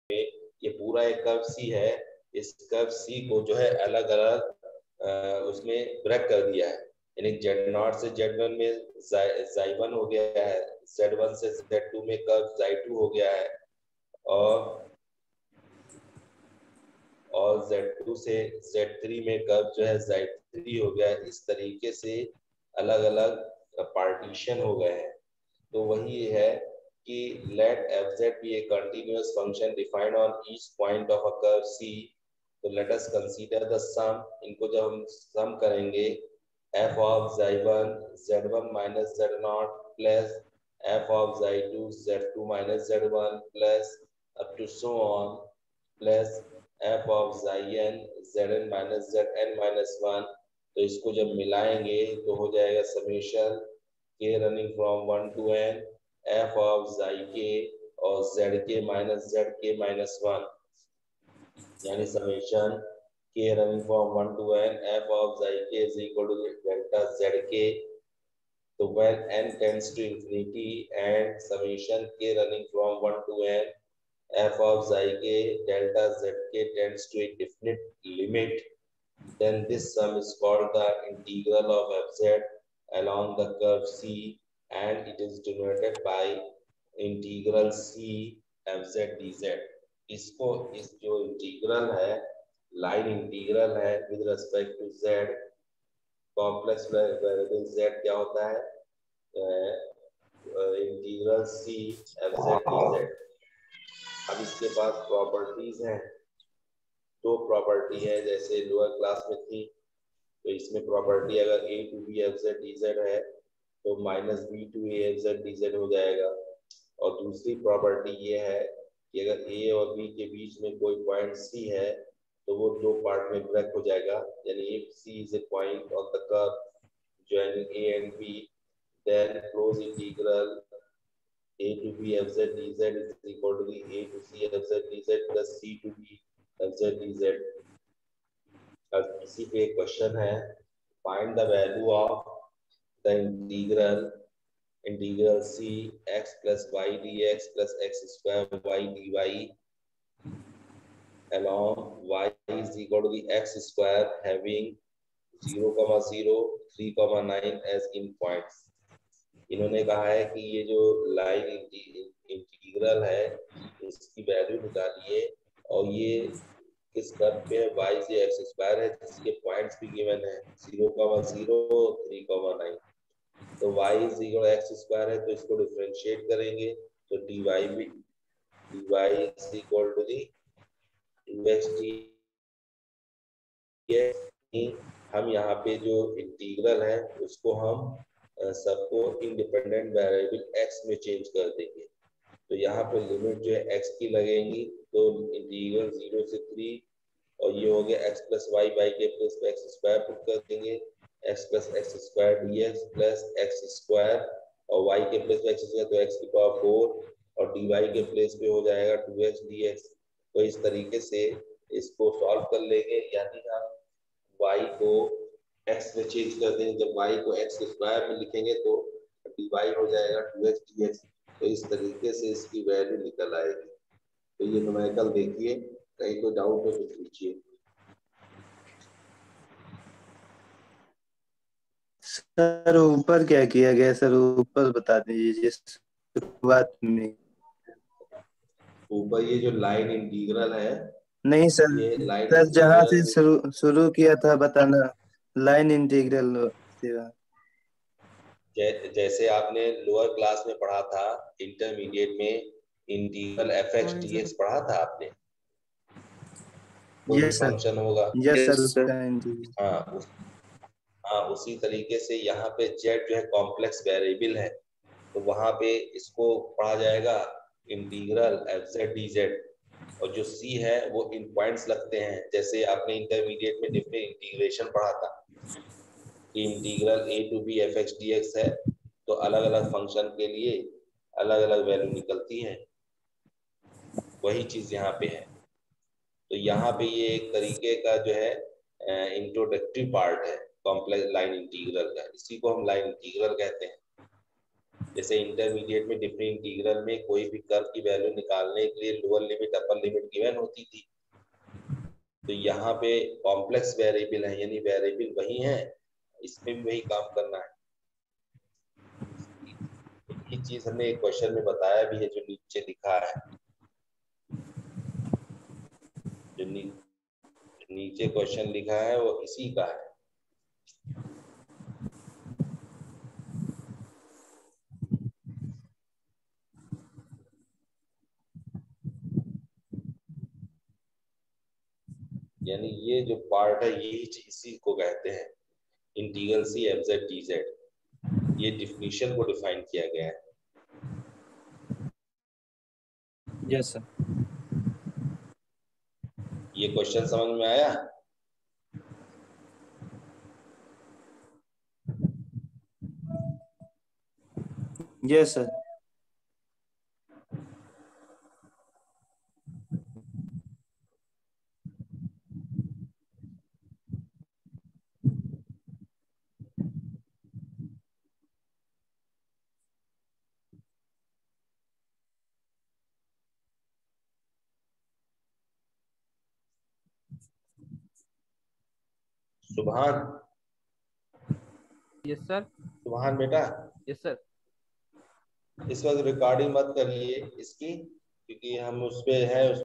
Line Integral wrt Complex Var Z & Cauchy Integral Theorem by Dr Shavej Ali Siddiqui 25-Apr-2021 Pt 1
Engineering Mathematics II lecture on 'Line Integral w.r.t Complex Variable Z & Cauchy Integral Theorem ' by Dr. Shavej Ali Siddiqui B.Tech 2nd Semester 25-April-2021 Part 1  || Khwaja Moinuddin Chishti Language University, Lucknow

Hello Everyone, this is the Maths Lecture on Topic 'Line Integral w.r.t Complex Variable Z & Cauchy Integral Theorem' of Engineering Mathematics II by Dr. Shavej Ali Siddiqui (Asst. Prof. KMCLU, Lucknow ) published on the 25th of April 2021

{-- Captions--}